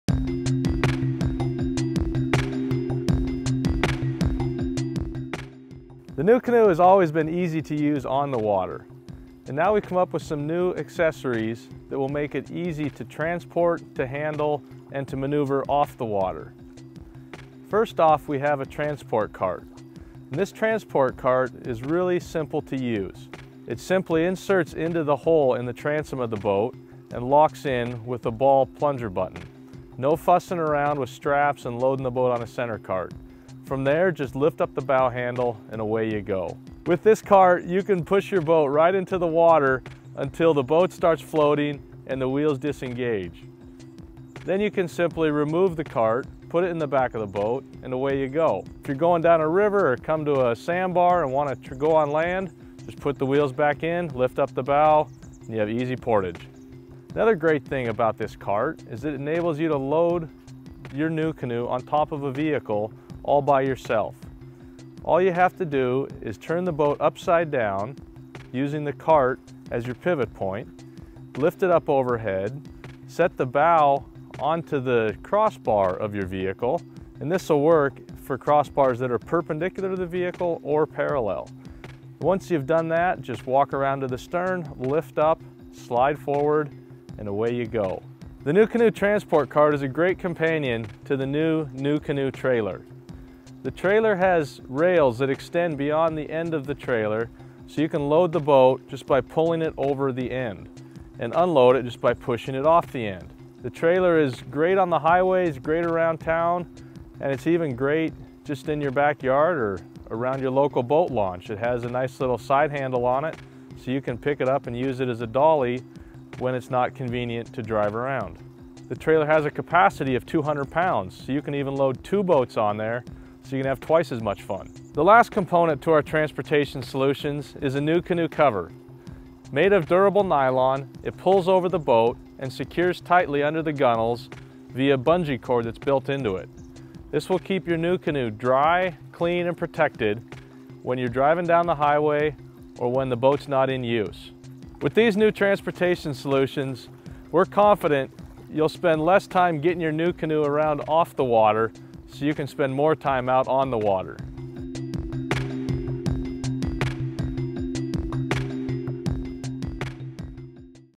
NuCanoe Transportation Solutions
The NuCanoe has always been easy to use on the water.  Now it is just as easy to move, transport, and store off the water.